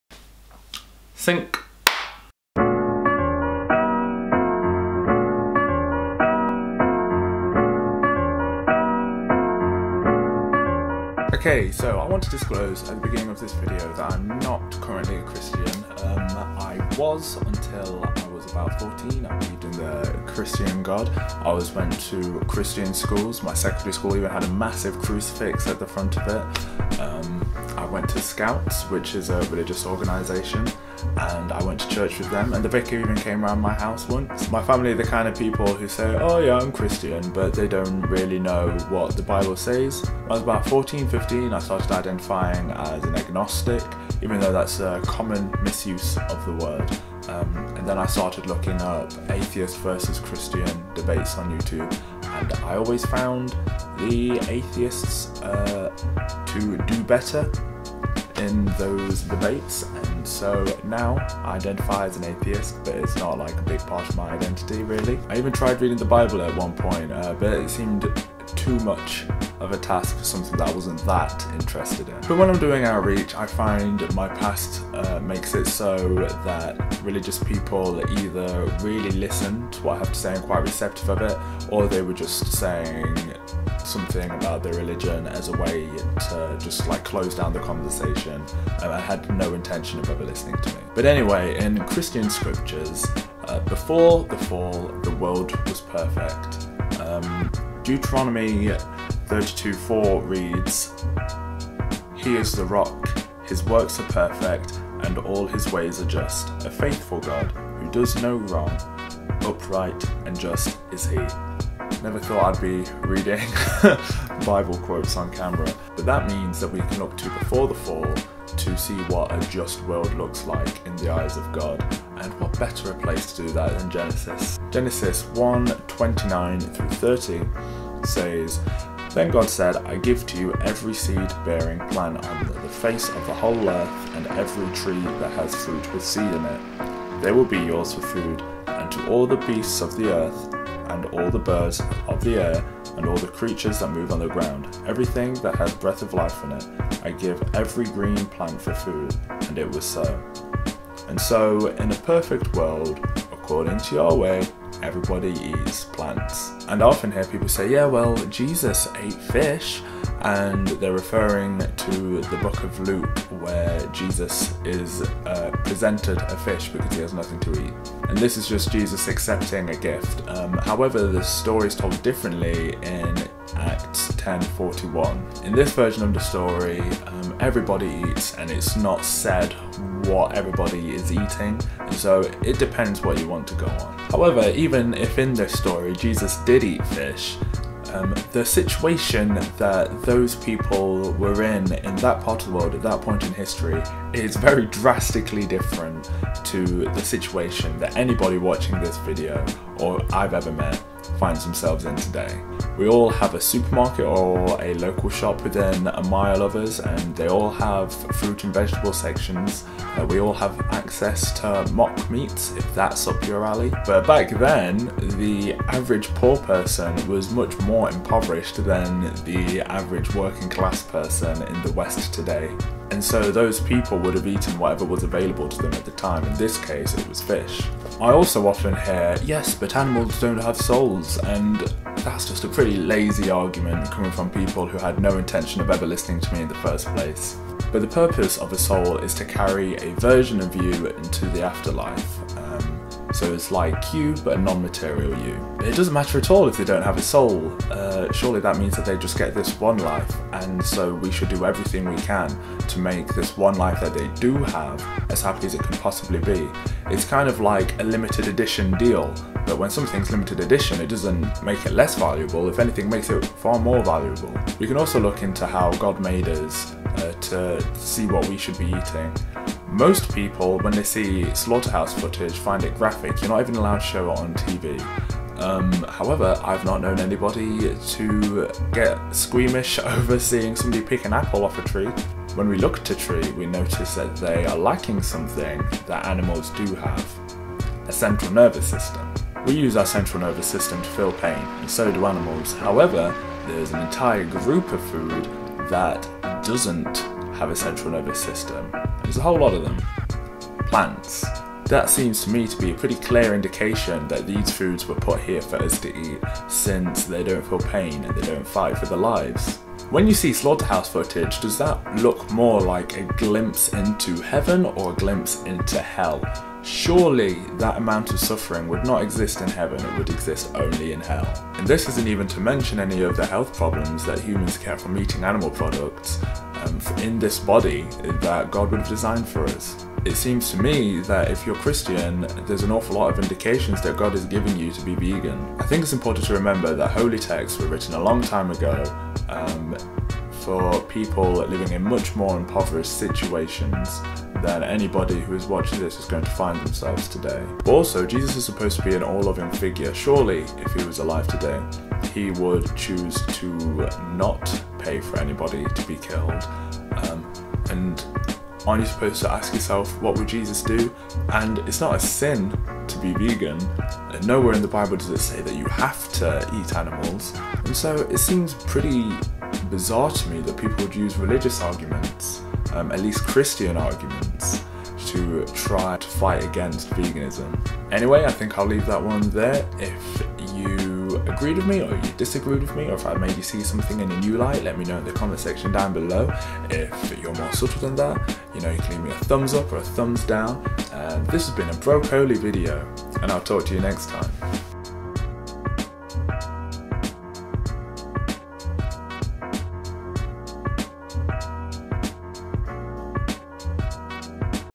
"But Jesus Ate Meat" • BRO COLI •
I often hear people use Christian arguments to try to argue against veganism but I've always felt like Christianity and veganism are perfectly compatible.

To stop the video being too long I didn't go into "but we have dominion over animals". I think that having dominion over animals would imply that we are to look after them and preserve this planet with their environments. In the same way that adults have dominion over children. Adults are supposed to care for children, not eat them- and it should be the same for non-human animals.